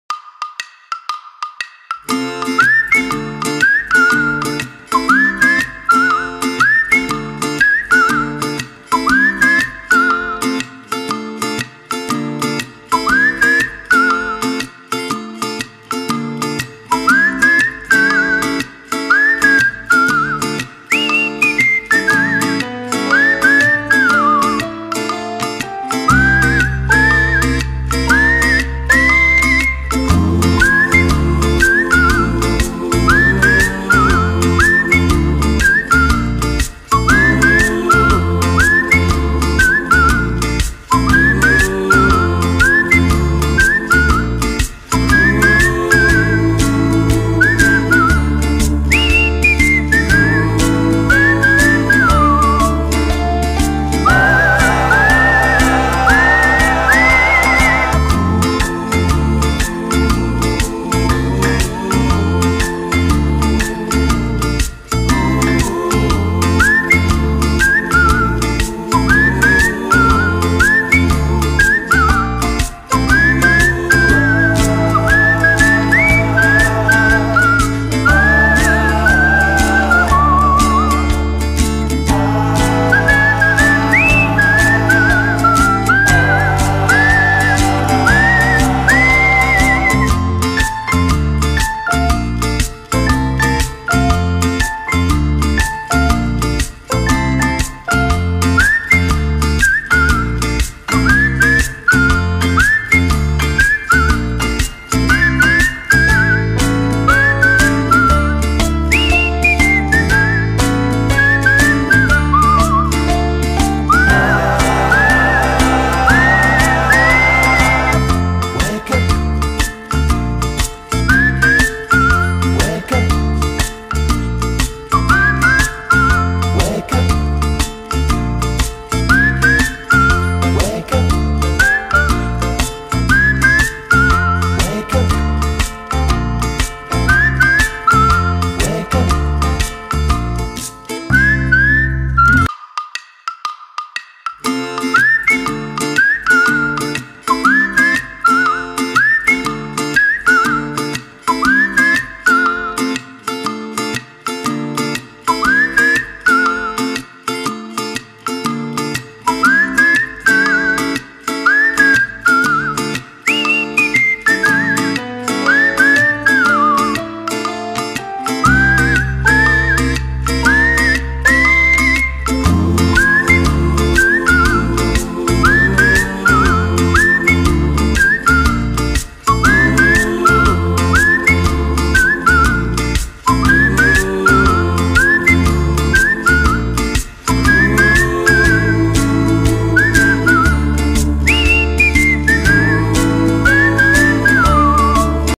Bird Nest In Banana Tree
Beautiful bird nest in my Banana tree
hope you are enjoying